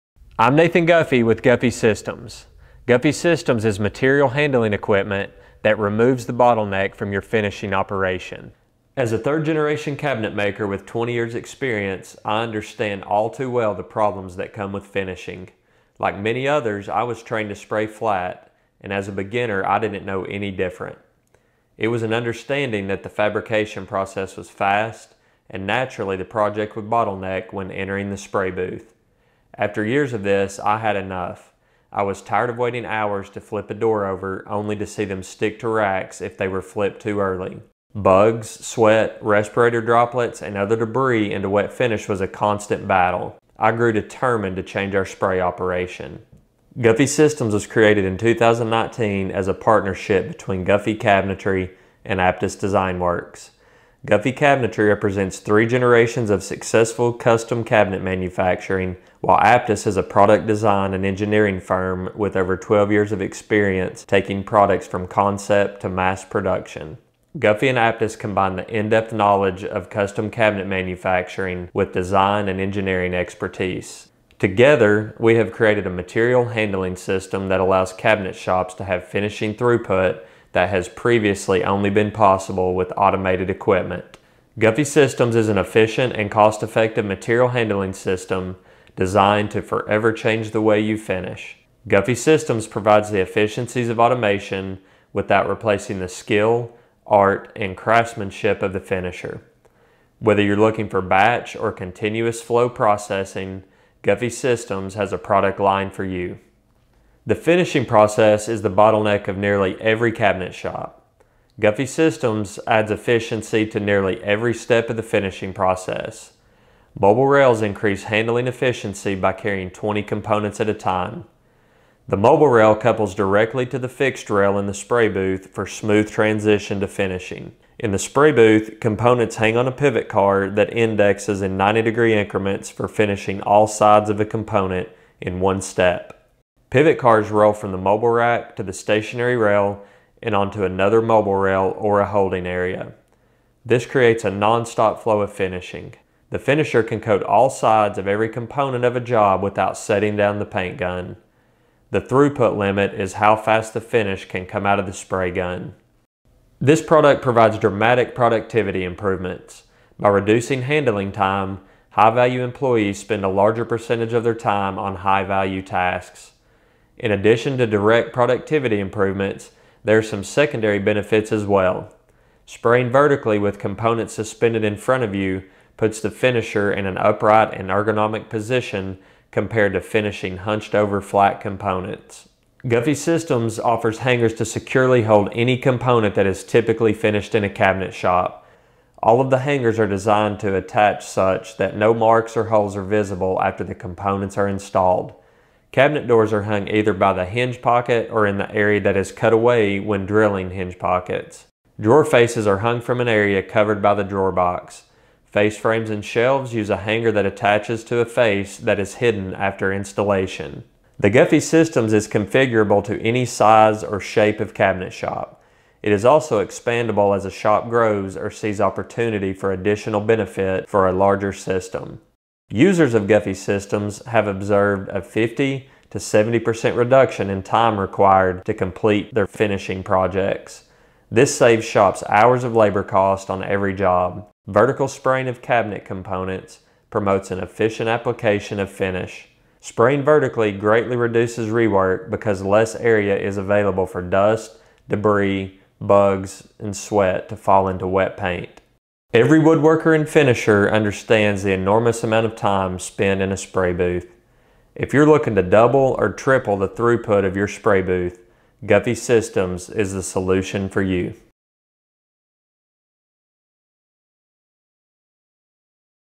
Guffey Systems Cabinet Finishing Products Intro
Nathan Guffey of Guffey Systems shares an overview of the company and our products. We offer material handling solutions for finishing any wood products. From bare wood to final coat, Guffey Systems products offer cost-effective solutions for all sizes of shops.